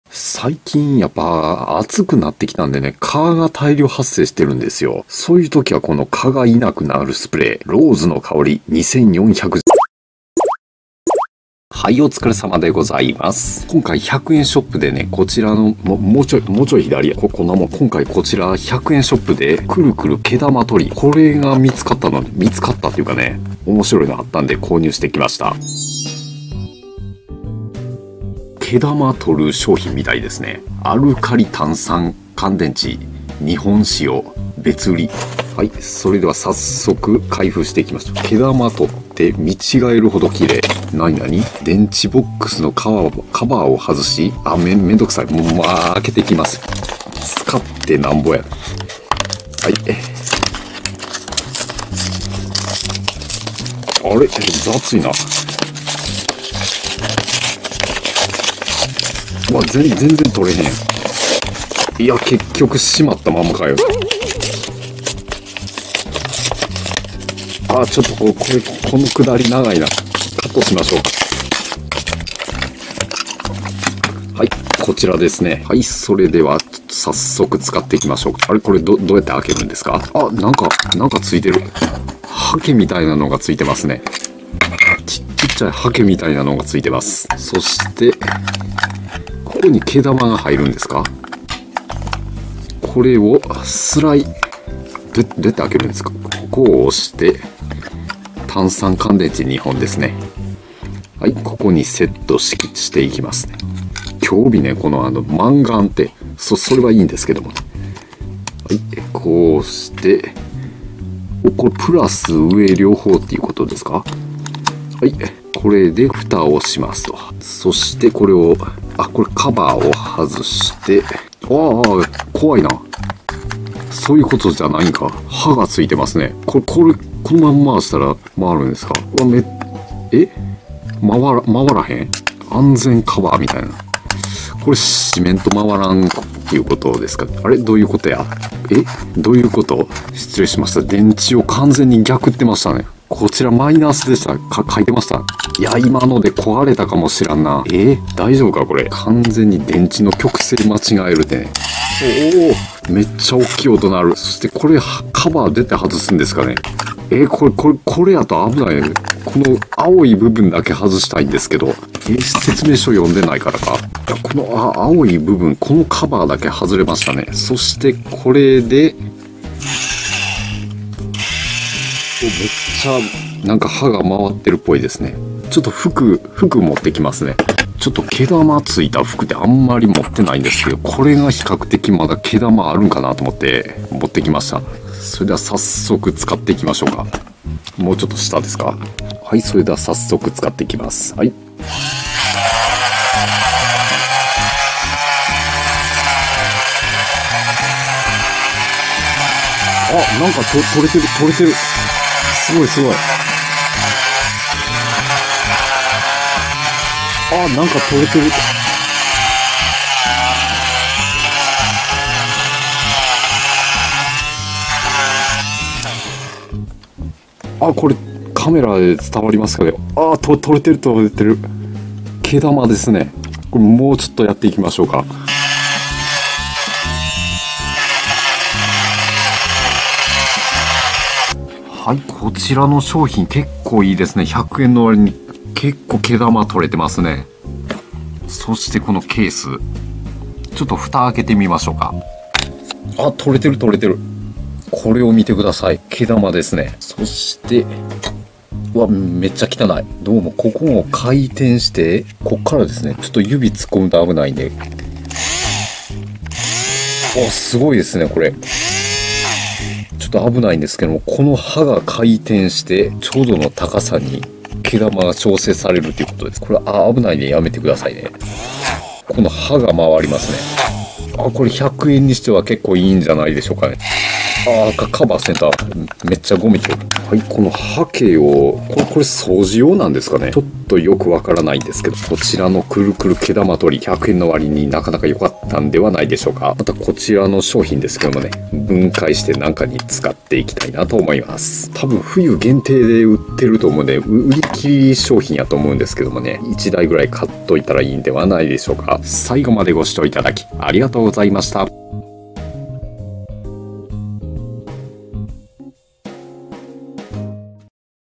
やてみた 150■100円 ダイソー毛玉取りは結構使える
冬限定で売っている商品。おそらく。
服を洗濯すると徐々に毛玉ができてくることがあります。
この商品は毛玉を簡単に取れるアイテムです。
単三×２本で駆動し、襟元や袖も楽々、毛玉が取れます。
取った毛玉はカートリッジに収納され、カチャっとはずして簡単に捨てられます。
掃除ようのハケも付属しており、値段が１００円。
フィギュアスケートでいうと、３回転トウループに匹敵します。